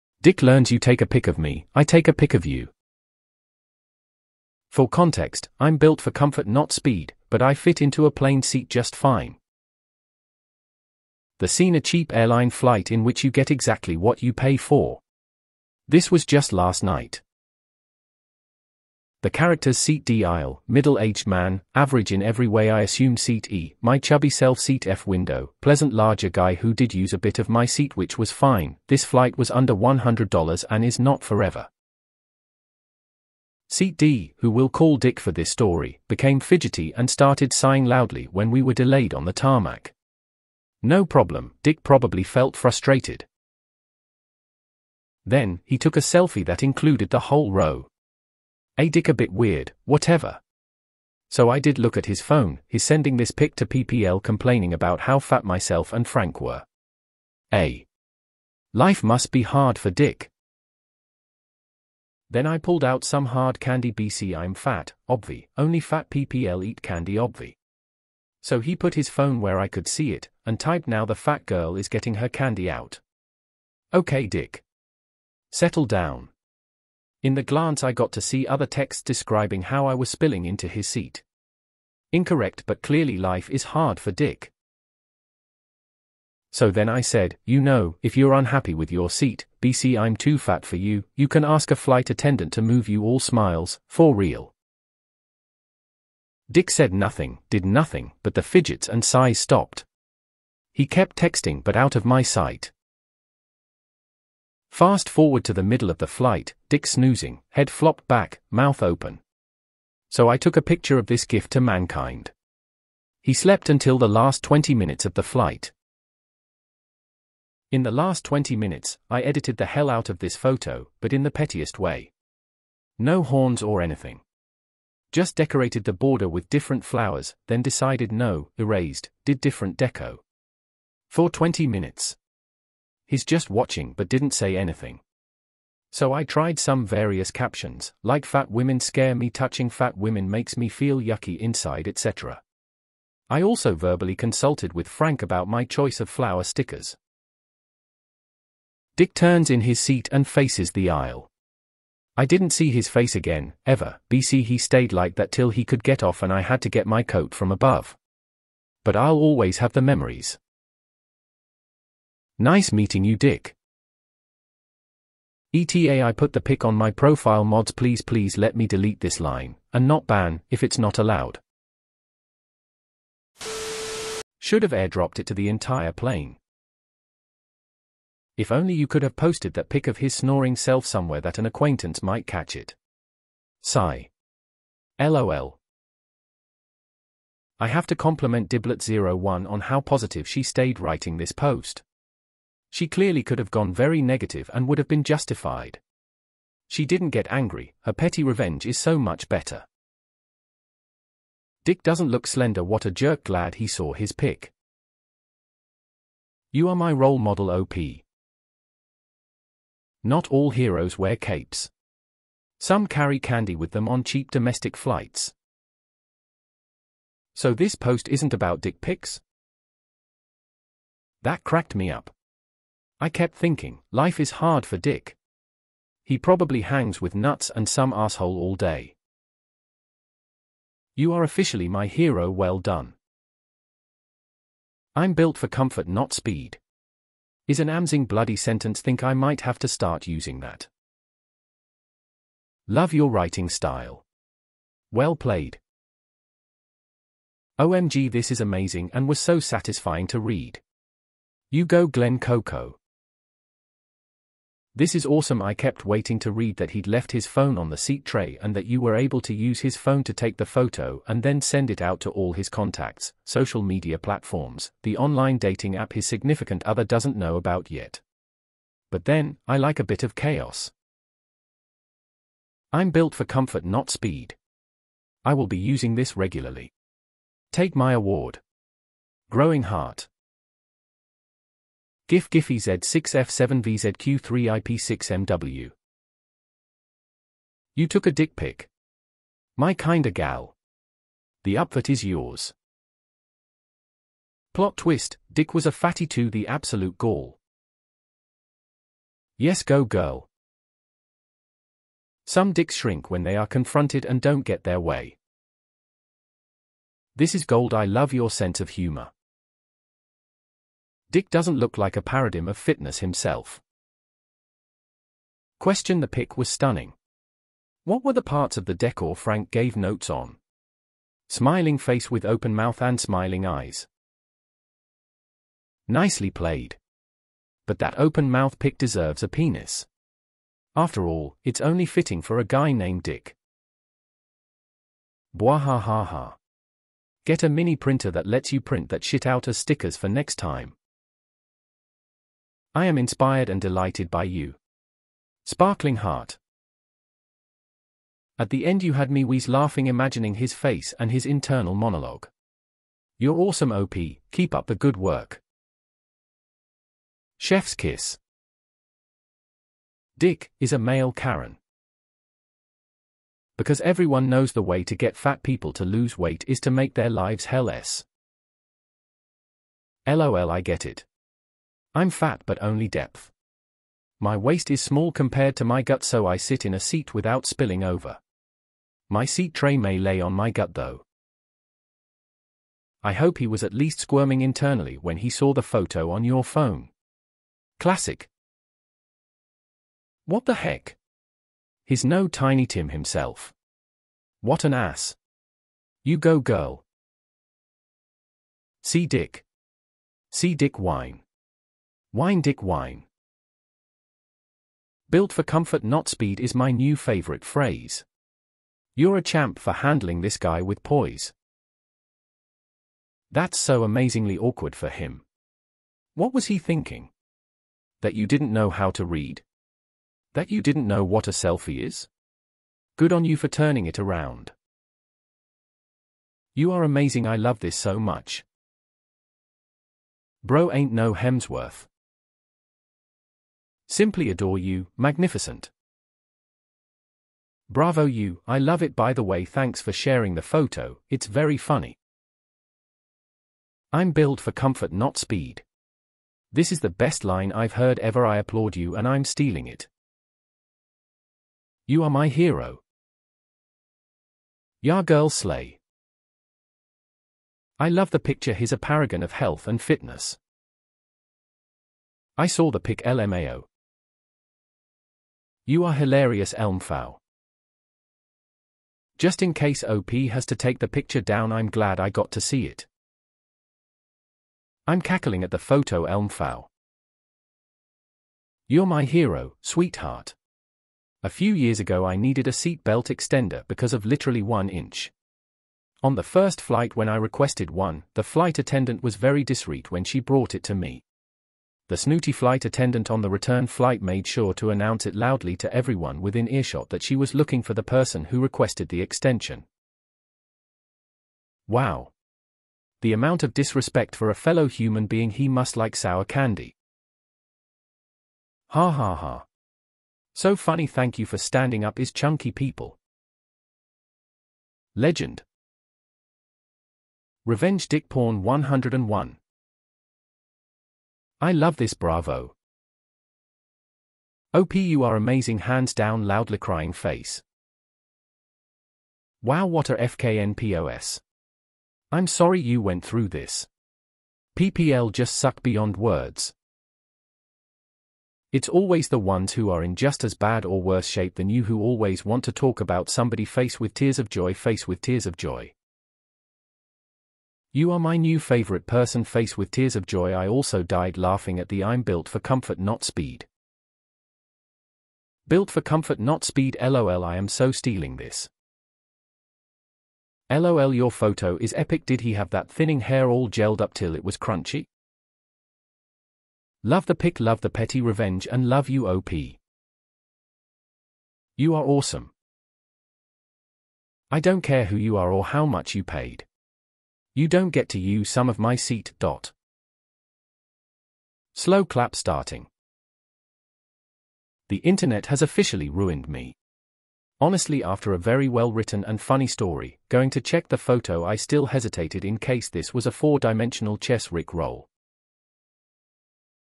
Dick learns you take a pic of me, I take a pic of you.
For context, I'm built for comfort not speed, but I fit into a plane seat just fine.

The scene: a cheap airline flight in which you get exactly what you pay for. This was just last night.

The characters: Seat D (aisle), middle aged man, average in every way (I assume); Seat E, my chubby self; Seat F (window), pleasant larger guy who did use a bit of "my seat" (which was fine, this flight was under $100 and is not forever).

Seat D, who we'll call Dick for this story, became fidgety and started sighing loudly when we were delayed on the tarmac. No problem, Dick probably felt frustrated.

Then, he took a selfie that included the whole row. Eh Dick a bit weird, whatever. So I did look at his phone, he's sending this pic to ppl complaining about how fat myself and "Frank" were. Eh. Life must be hard for Dick.

Then I pulled out some hard candy (bc I'm fat, obvi, only fat ppl eat candy obvi). So he put his phone where I could see it, and typed "now the fat girl is getting her candy out". Ok Dick. Settle down. In the glance I got to see other texts describing how I was spilling into his seat. Incorrect but clearly life is hard for Dick.

So then I said, "you know, if you're unhappy with your seat, bc I'm too fat for you, you can ask a flight attendant to move you :)" (all smiles, for real).

Dick said nothing, did nothing, but the fidgets and sighs stopped. He kept texting but out of my sight.

Fast forward to the middle of the flight, Dick's snoozing, head flopped back, mouth open. So I took a picture of this gift to mankind. He slept until the last 20 min of the flight.

In the last 20 min, I edited the hell out of this photo, but in the pettiest way. No horns or anything. Just decorated the border with different flowers, then decided no, erased, did different deco. For 20 min. He's just watching but didn't say anything. So I tried some various captions, like "fat women scare me" "touching fat women makes me feel yucky inside" etc. I also verbally consulted with Frank about my choice of flower stickers.

Dick turns in his seat and faces the aisle. I didn't see his face again, ever, bc he stayed like that till he could get off and I had to get my coat from above.  But I'll always have the memories.

Nice meeting you Dick.

ETA: I put the pic on my profile (mods please please let me delete this line, and not ban, if it's not allowed)